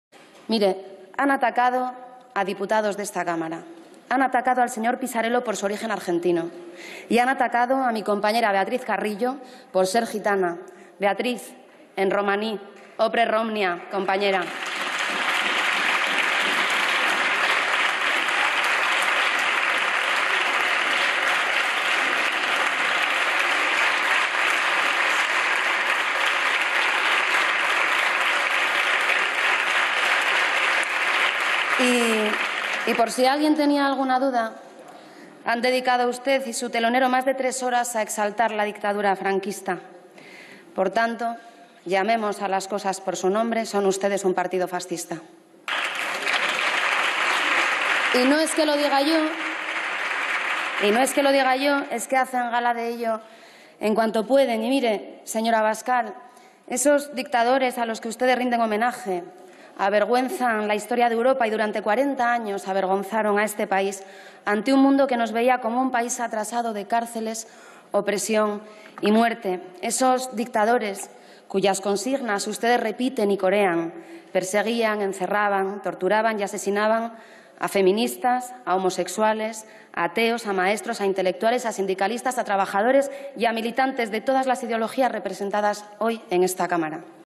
Adriana Lastra, a Vox: "Son un partido fascista"
A Santiago Abascal se ha referido para decir: "Llamemos a cada cosa por su nombre; son ustedes un partido fascista y hacen gala de ello cuando pueden". También les ha acusado de convertir el Congreso en un "circo de ruido y furia": "Vaya teatrillo se ha montado". Al resto de partidos les ha pedido "hablar de manera urgente del desafío" que supone la extrema derecha a la democracia.